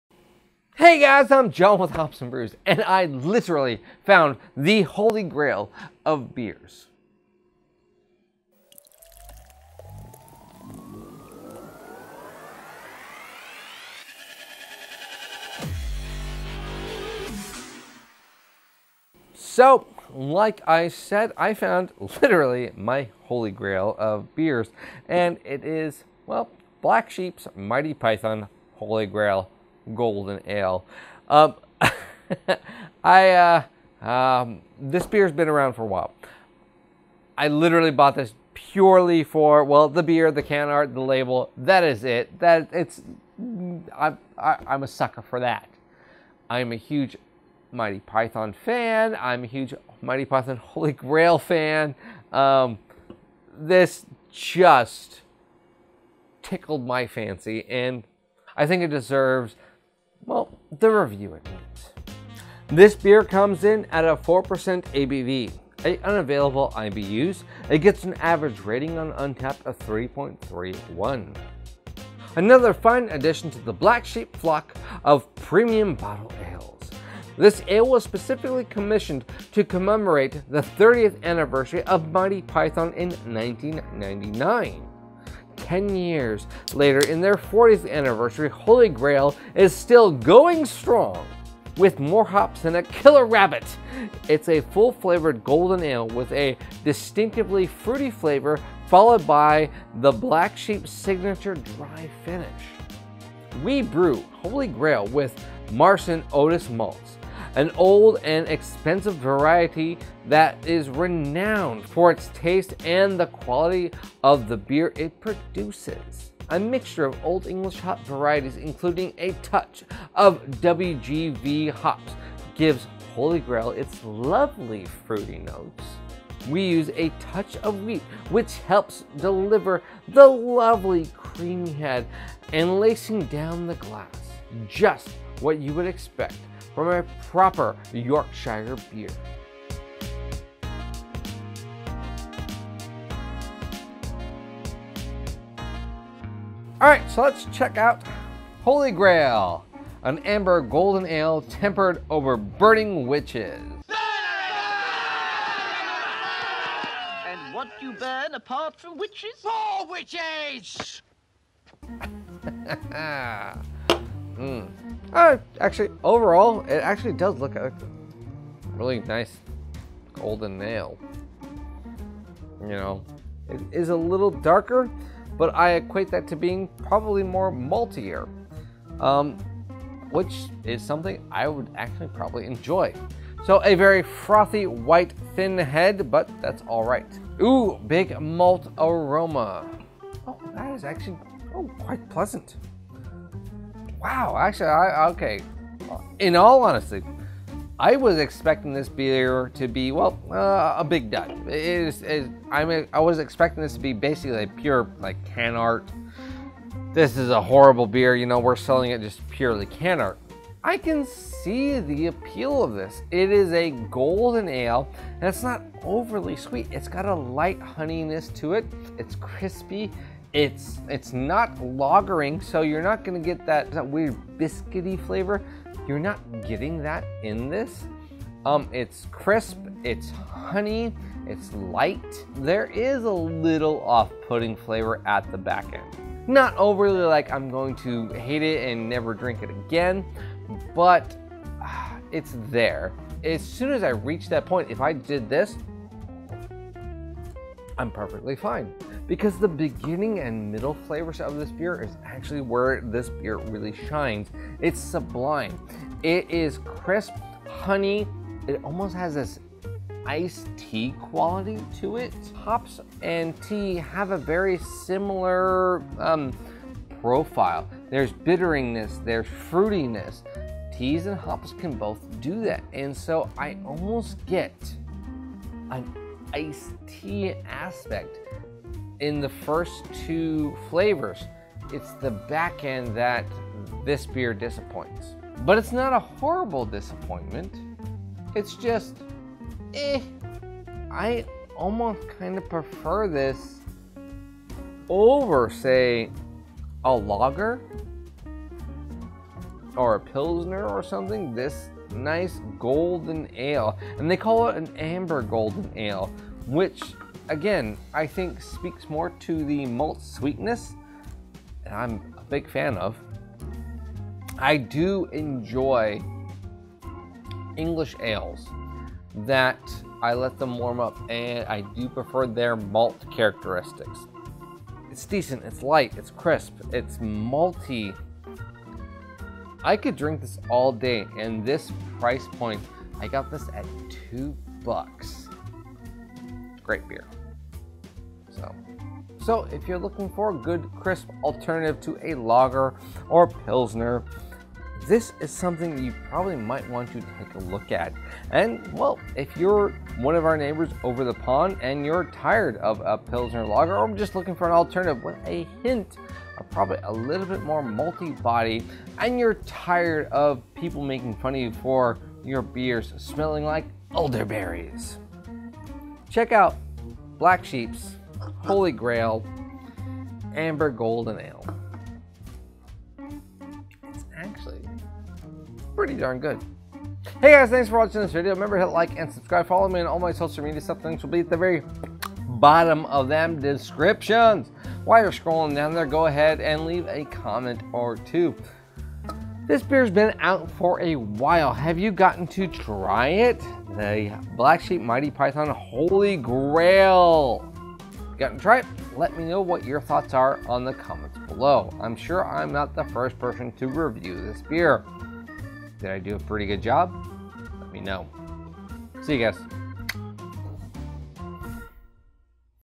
Black Sheep Brewery - Monty Python's Holy Grail
Glass Cleaning

BEER NAME: Black Sheep Brewery - Monty Python's Holy Grail
ABV: 4.0%
IBU: N/A

Profile: "The quest is over. It’s undeniable that British humour and great beer go well together, so the flock here at the Brewery and the team at Monty Python joined forces to launch the Holy Grail.

Famously “tempered over burning witches”, Holy Grail was originally created to commemorate the 30th anniversary of Monty Python, and is a much-loved part of our bottled beer collection. Some 19 years on, The beer has been given a new lease of life with the recent reunion and the subsequent Spamalot tour.

Monty Python’s Holy Grail is a light golden beer with a fresh, zesty taste and a long crisp bitter finish. It’s brewed with a mixture of hops, including WGV, which gives the beer a light fruity nose. It’s a real tongue pleaser.

“Your mother was a hamster and your father smells of elderberries!”"